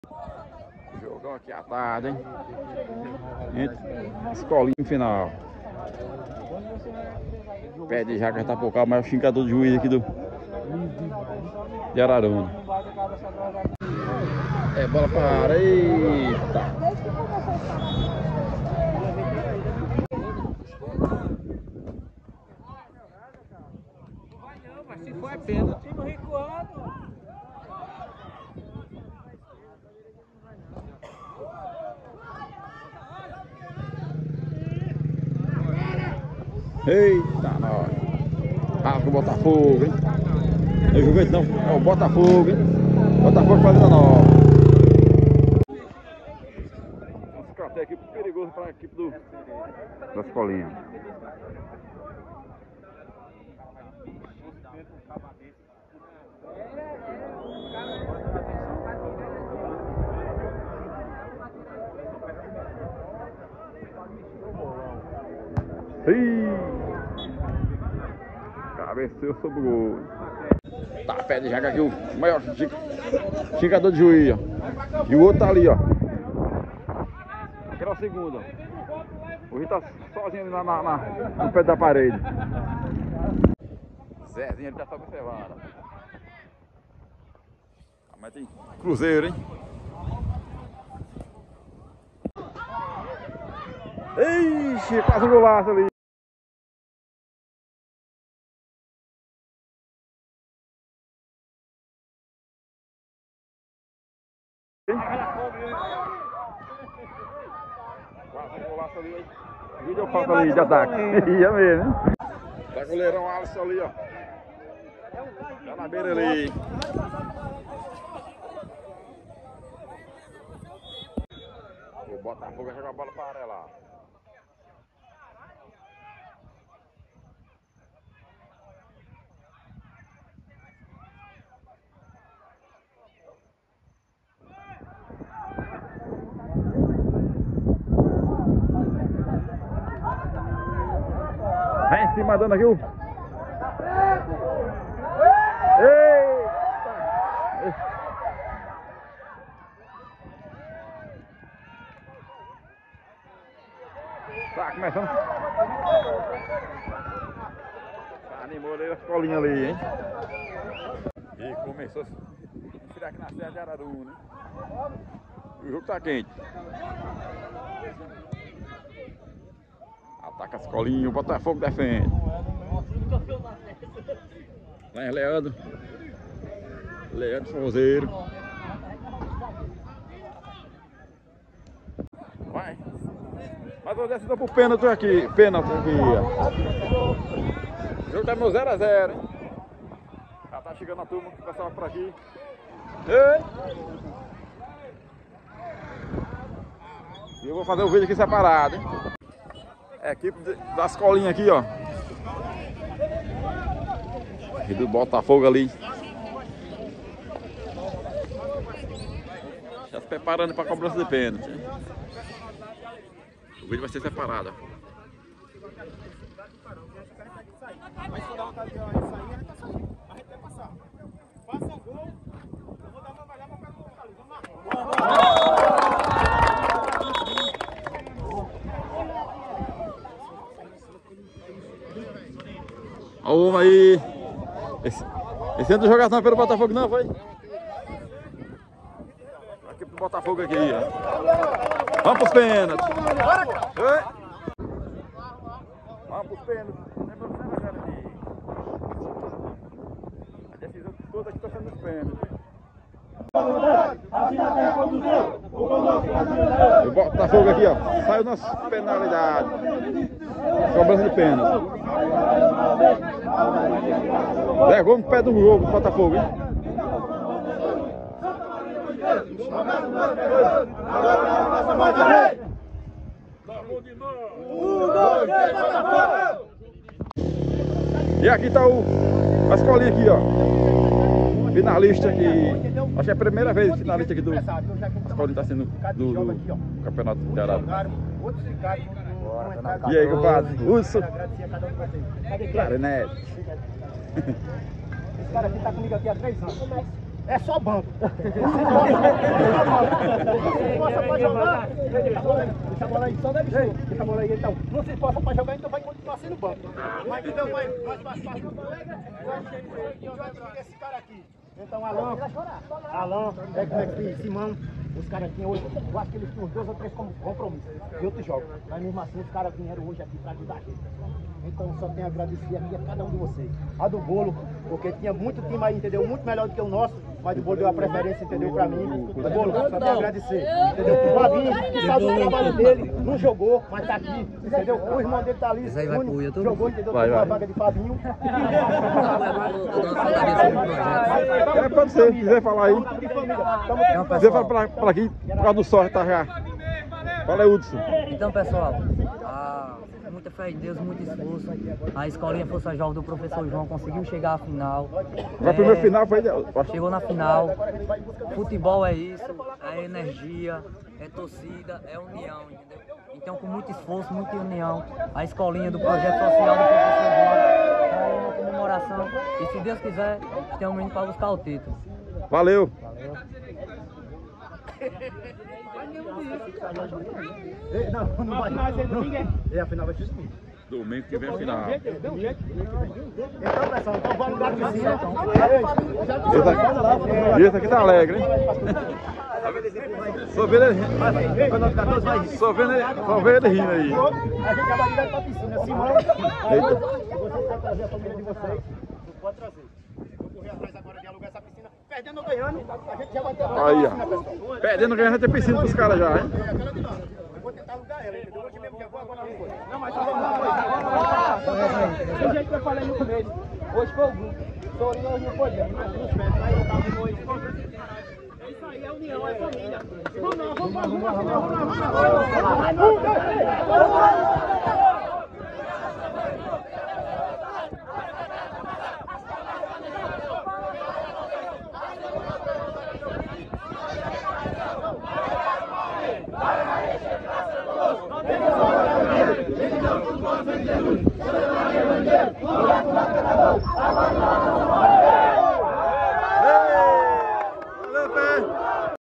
Um bom público foi assistir a partida da semi final entre botafogo x escolinha no campo do Bernardo
Copa araruna araruna de futebol 2024